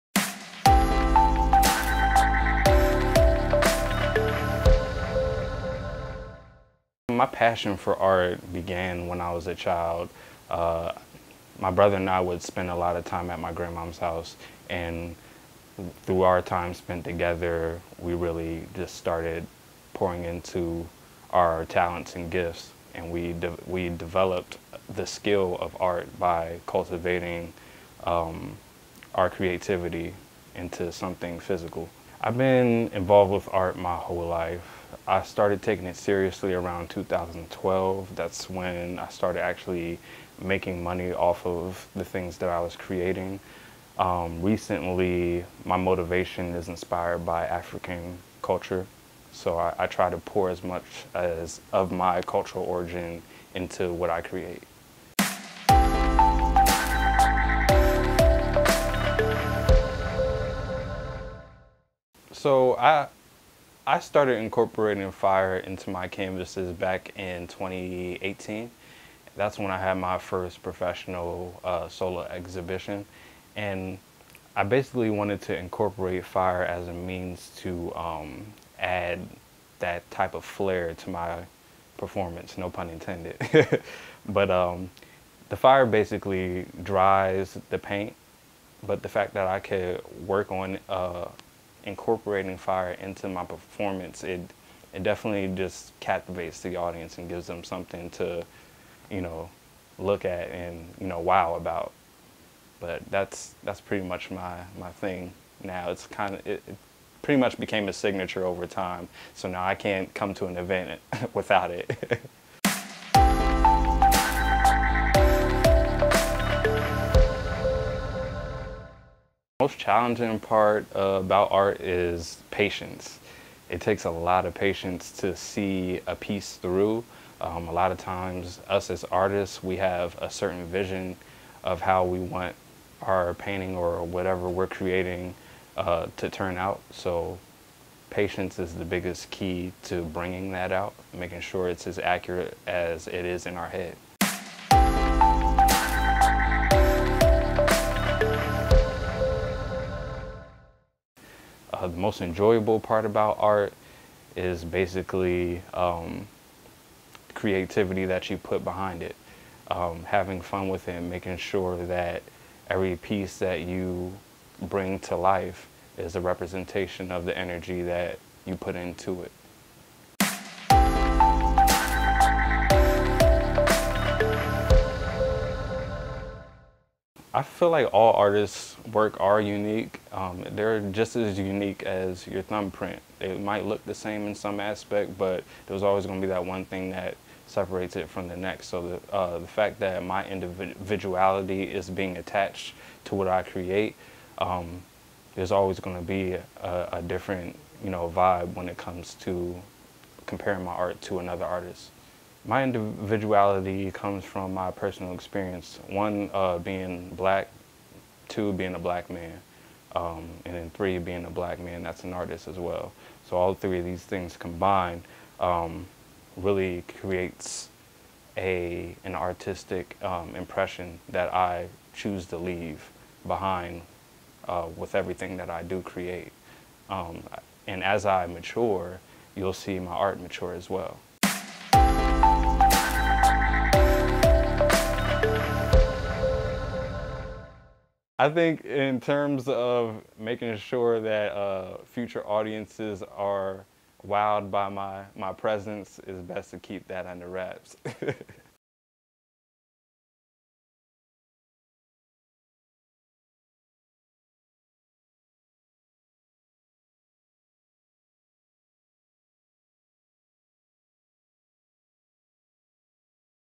Looking for some hot stuff? Check out this artist who sets his paintings on fire
Ahead of Saturday's  YPOC’s Midtown Paint and Sip event at Good Brothas Cafe, we had the chance to sit down with local artist "King Profilik." Watch to learn more about the King Prolifik and the inspiration behind his artwork.